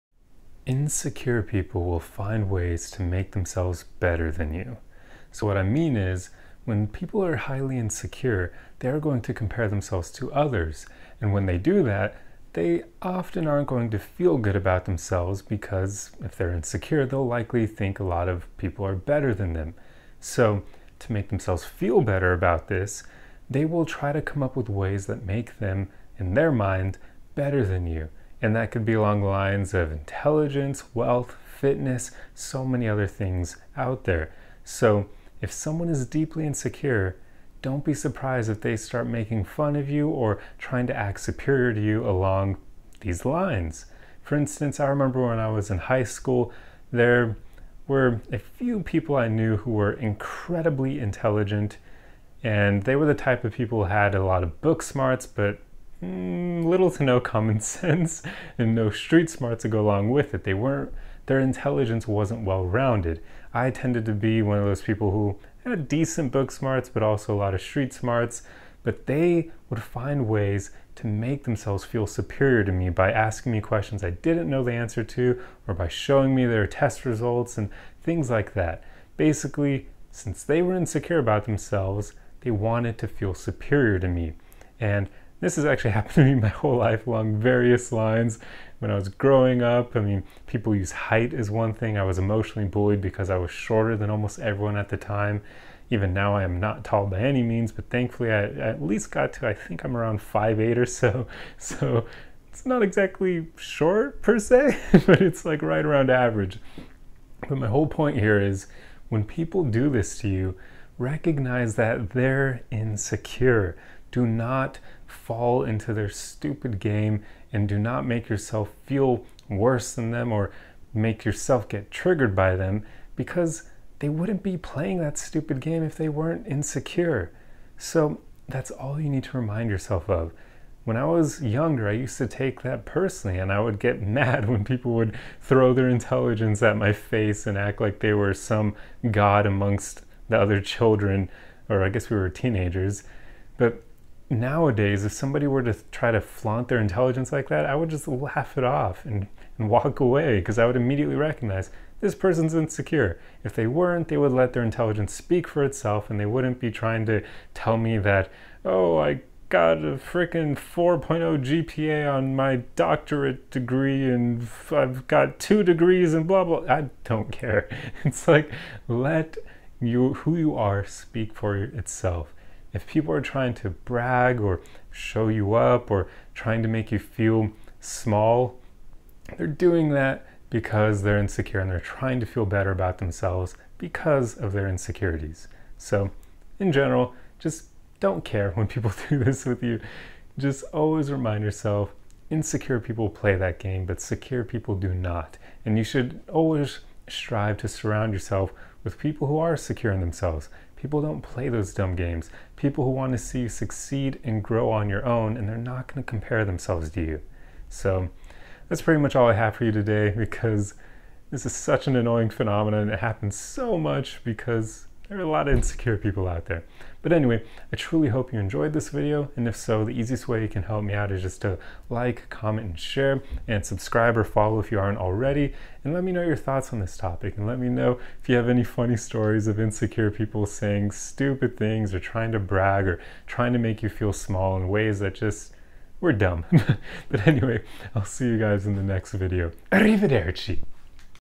Insecure People Will Find Ways to Be ‘Better’ Than You
Day 606. Insecure people will often play mind games to put you down and make themselves feel better than you. The best response is to stay grounded.